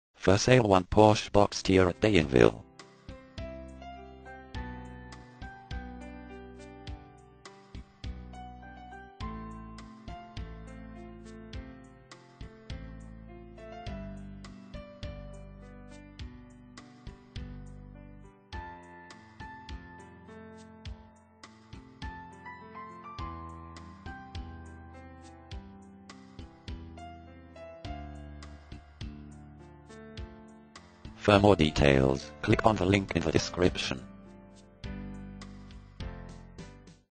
Sale one Porsche Boxster Dainville
For sale at  Dainville  Used car Porsche Boxster of 84000 Km.
used motors Autoenvideo : Sale one Porsche Boxster  Dainville  Pas-de-Calais